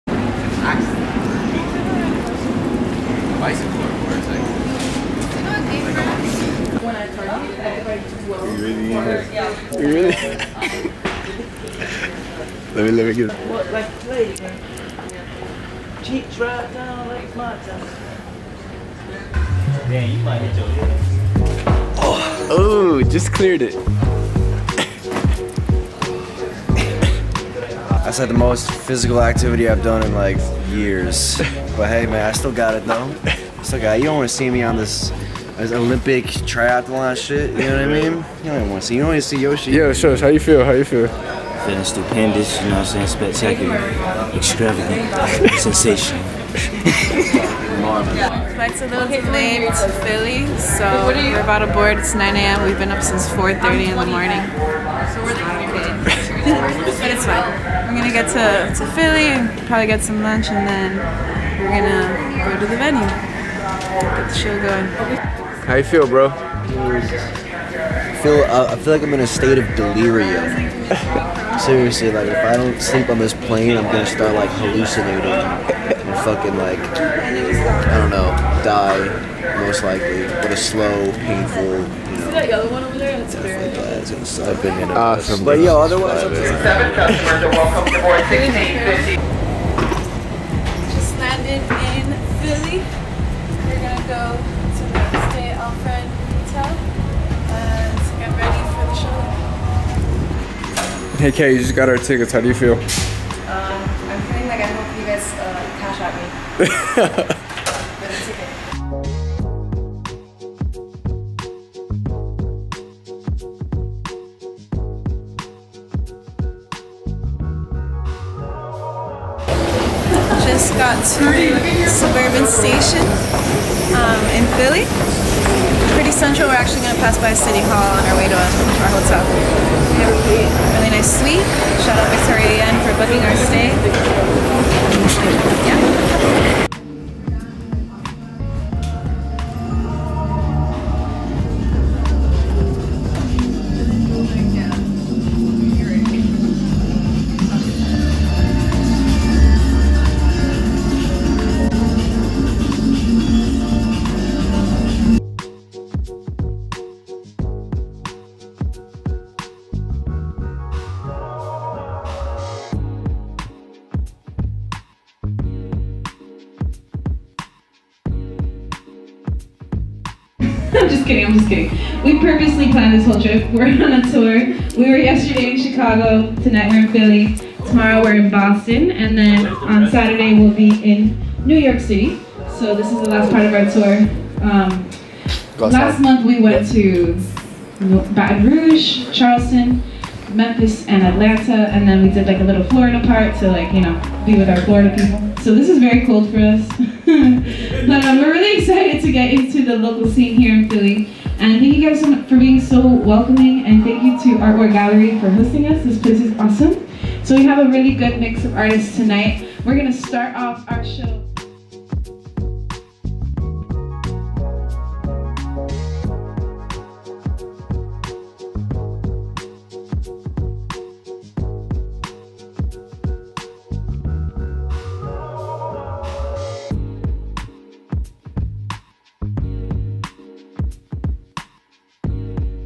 From Miami, With Love | Backroom Takes Over Philly
From Miami, With Love | Philly

Tag along for the ride as we go across four states in the last leg of our East Coast Tour. This is PHILLY! shot: @bausemason edit: @machtheshaman @crackdaniels @yoshithompkins @theartweargallery @_luna_lunera @katarinaalexiss @catinthewallband @nastyelgic @10d4ys @a1_x_lucas_ @lunabelunar @josie_agatha @alexanderpineda_ @cynvisualss